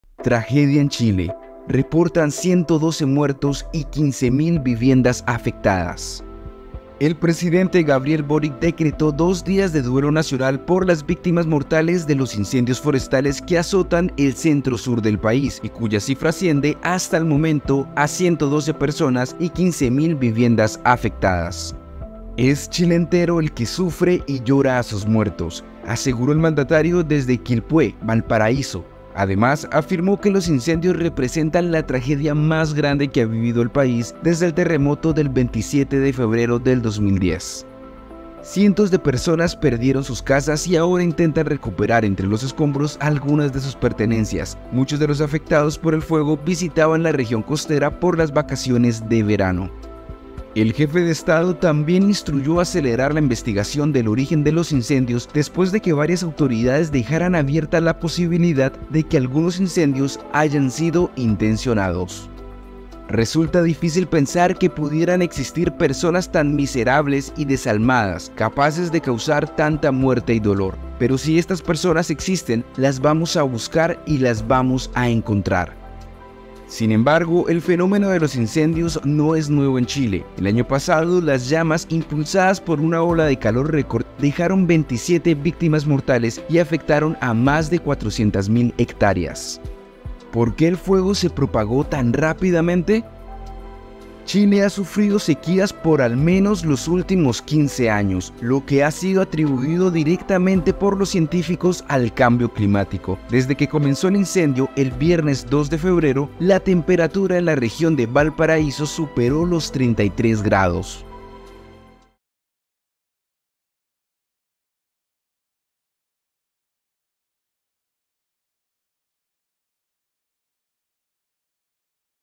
Tragedia en Chile: Reportan 112 muertos y 15.000 viviendas afectadas | El Diario
El presidente Gabriel Boric decretó dos días de duelo nacional por las víctimas mortales de los incendios forestales que azotan el centro-sur del país y cuya cifra asciende, hasta el momento, a 112 personas y 15.000 viviendas afectadas. Chile ha sufrido sequías por al menos los últimos 15 años, lo que ha sido atribuido directamente por los científicos al cambio climático. Desde que comenzó el incendio, el viernes 2 de febrero, la temperatura en la región de Valparaíso superó los 33 grados.